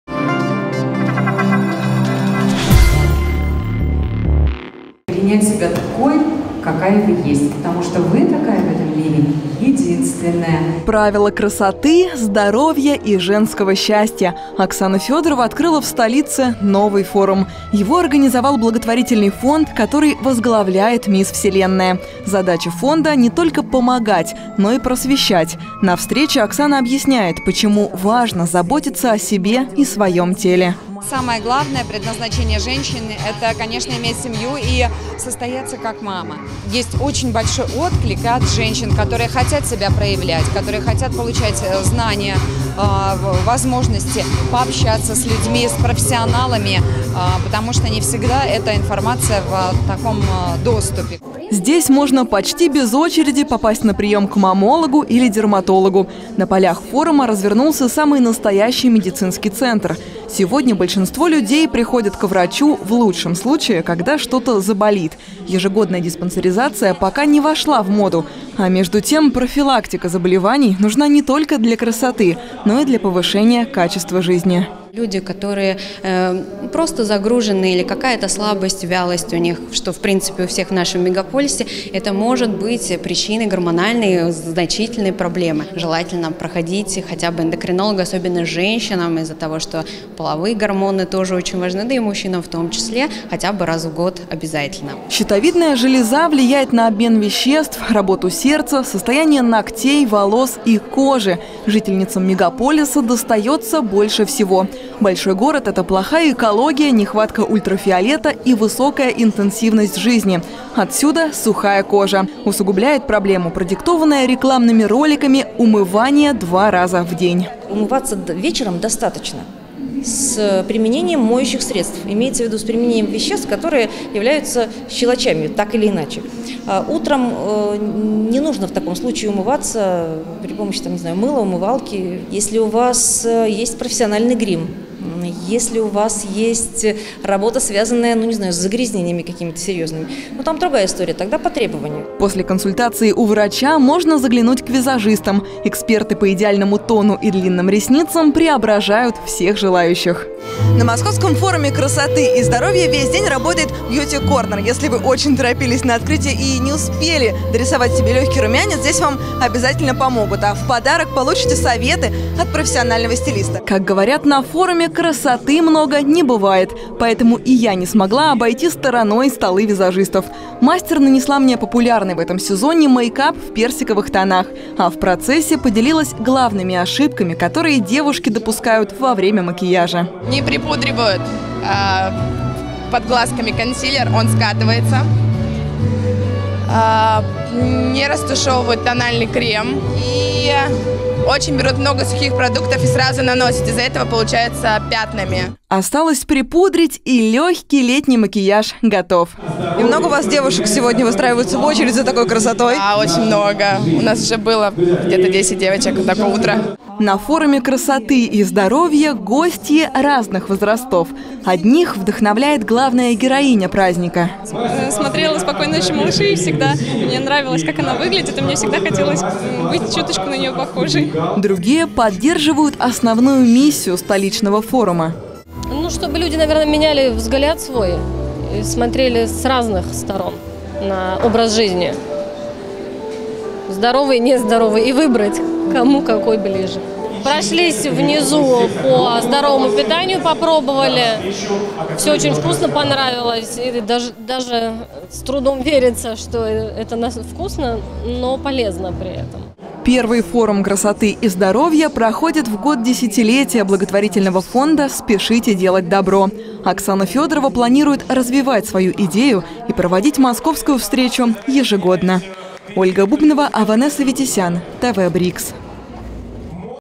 Московский форум красоты и здоровья
«Принять себя такой, какая Вы есть. Потому что Вы такая в этом мире единственная!».

Правила красоты, здоровья и женского счастья: Оксана Федорова открыла в столице новый форум. Его организовал благотворительный фонд, который возглавляет Мисс Вселенная. Задача фонда – не только помогать, но и просвещать. На встрече Оксана объясняет, почему важно заботиться о себе и своем теле.